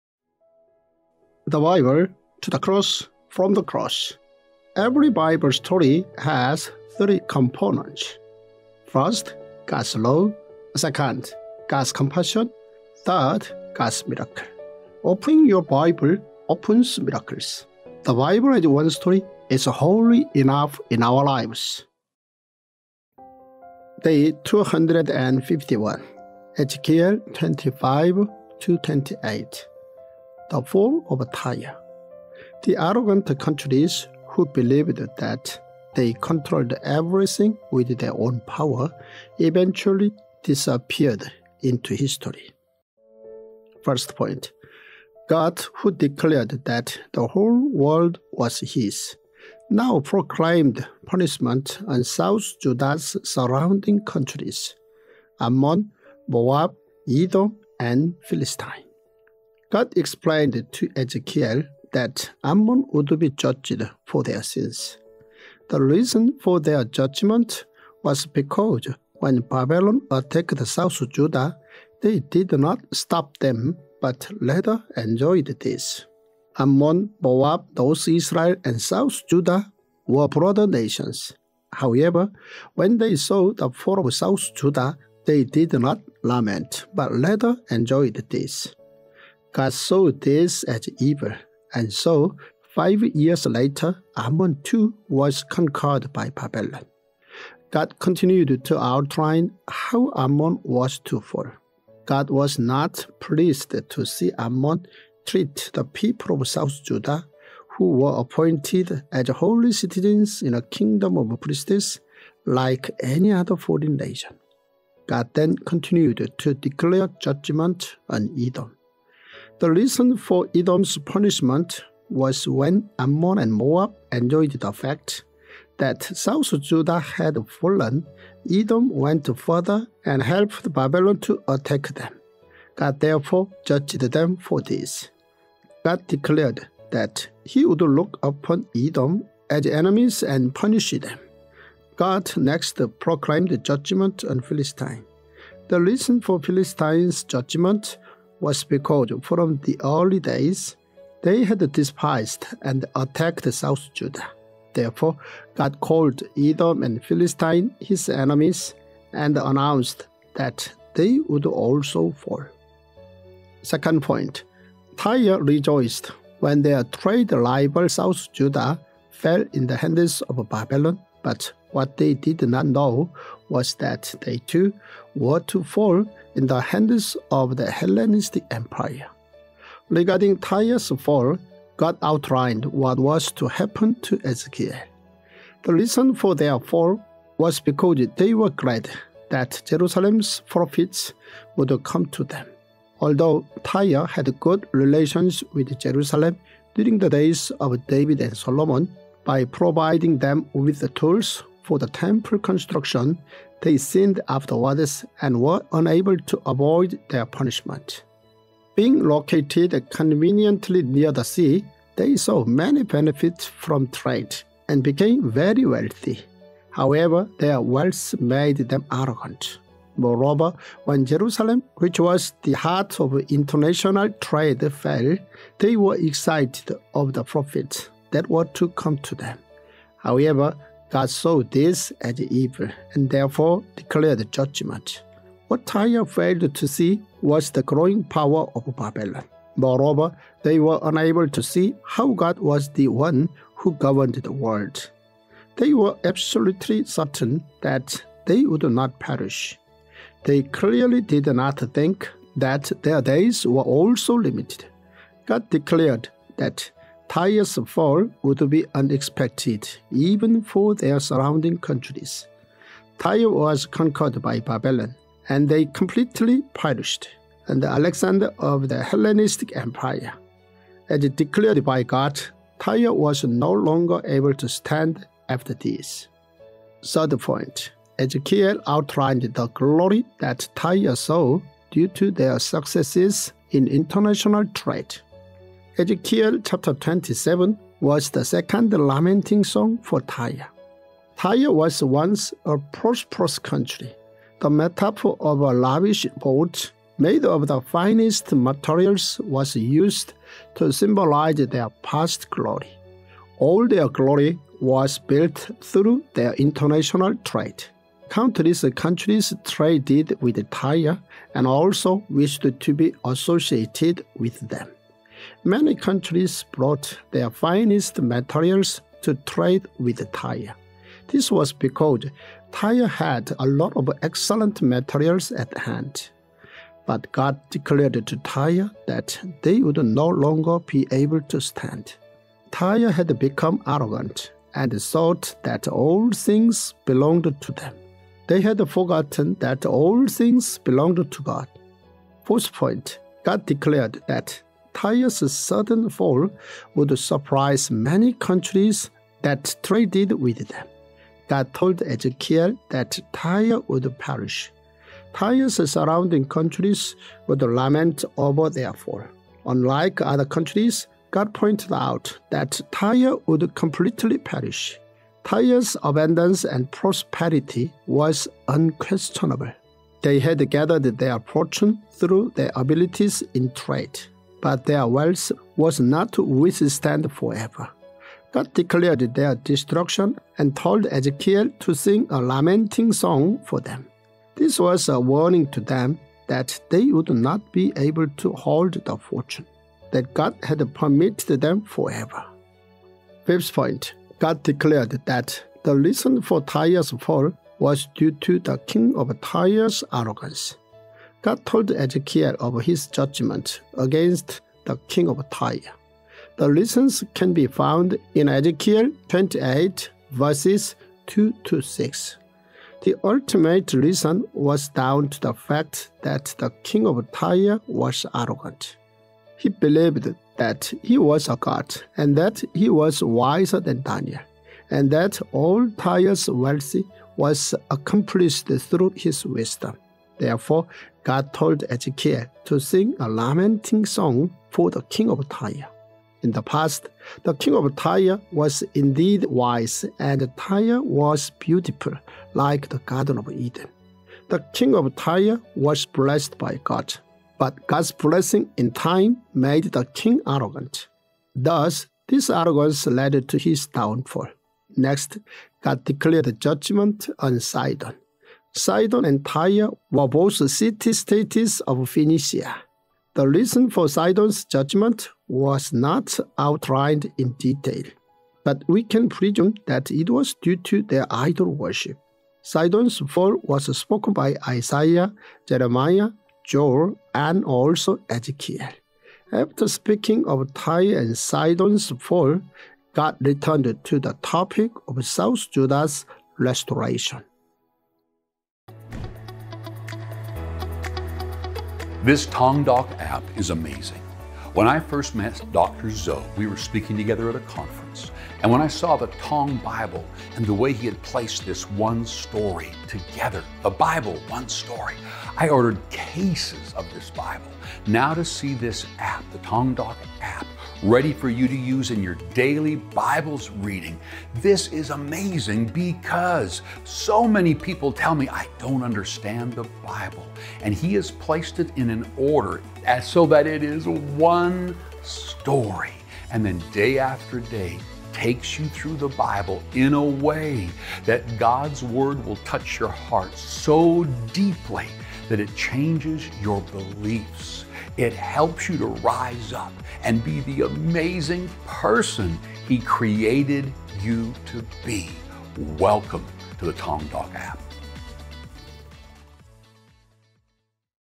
Tongdok Bible | Day 251 | Ezekiel 25-28 | The fall of Tyre | Dr.Byoungho Zoh
Tongdok Bible | Day 251 (Ezekiel 25-28)

The fall of Tyre
The arrogant countries who believed that they controlled everything with their own power eventually disappeared into history.